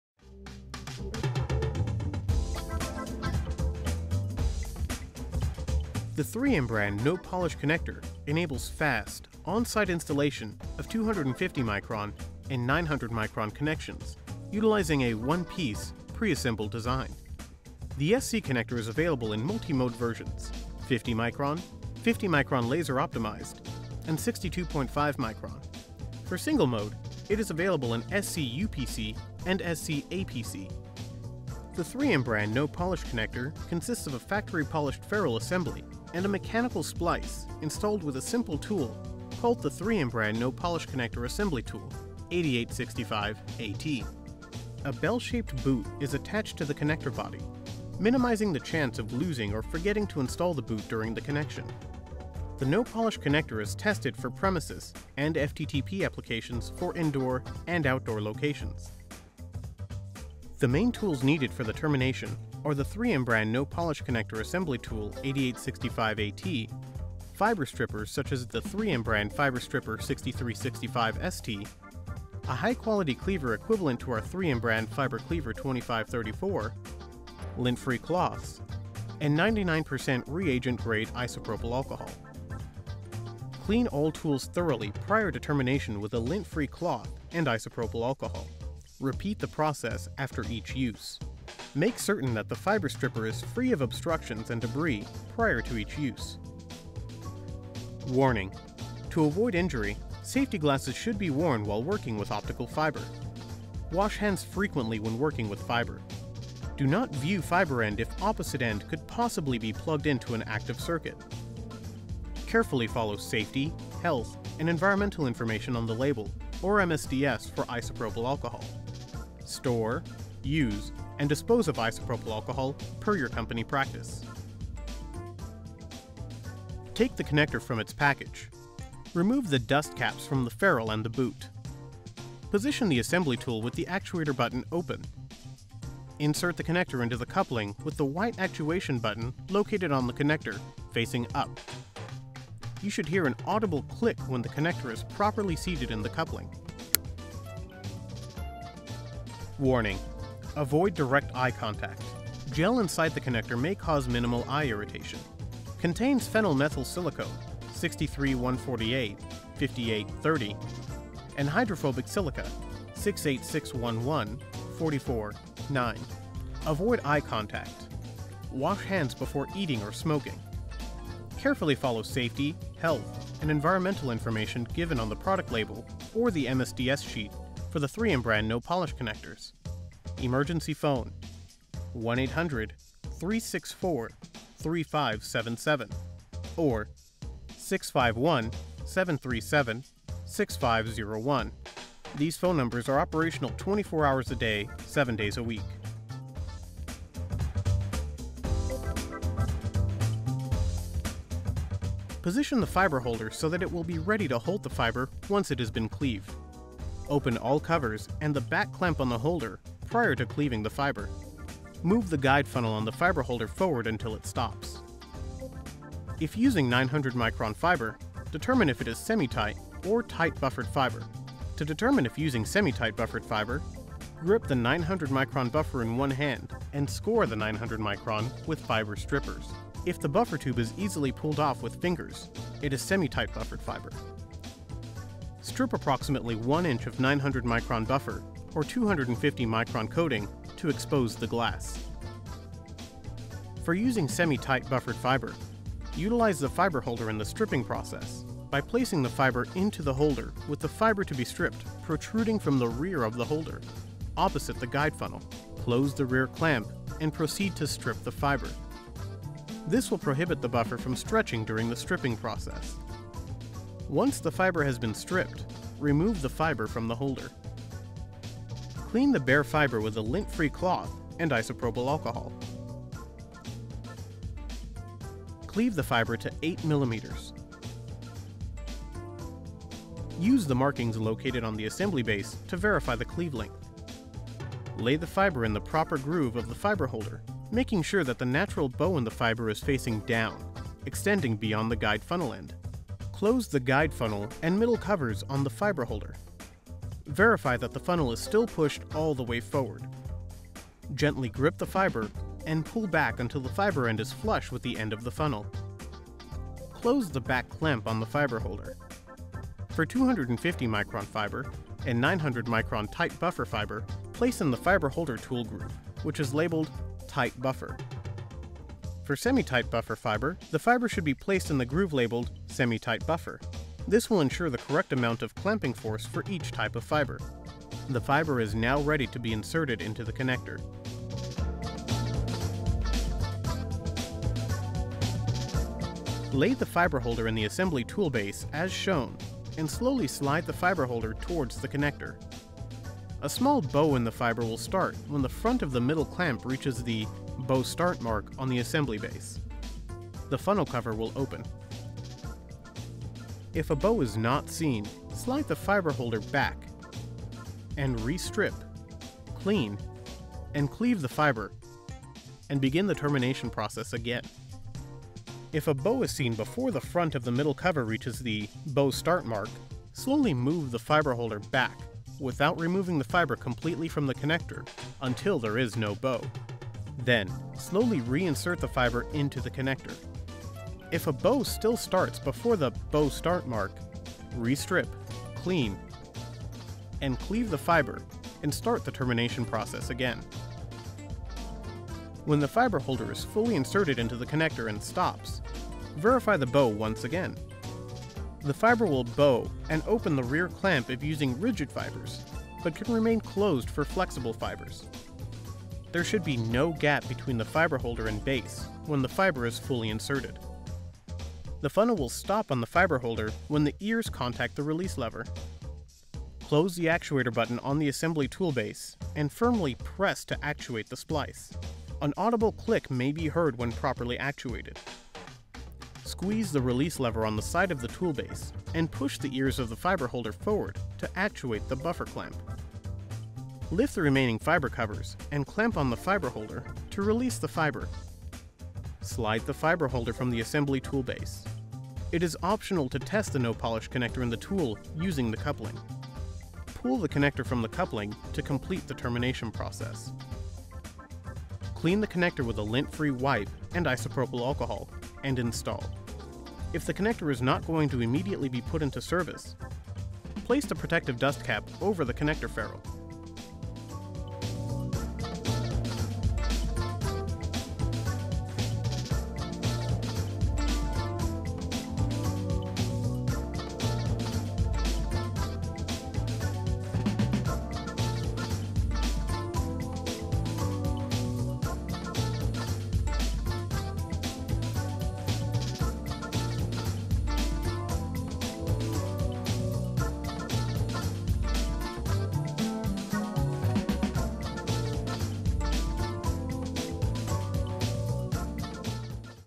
De glasvezel connector "No Polish Connector"
Super snel glasvezels afmonteren in slecht 2 minuten? U heeft geen ingewikkelde en kostbare tools meer nodig wanneer u kiest voor de No Polish Connector van 3M. Iedereen kan het en de kwaliteit spreekt voorzich!!!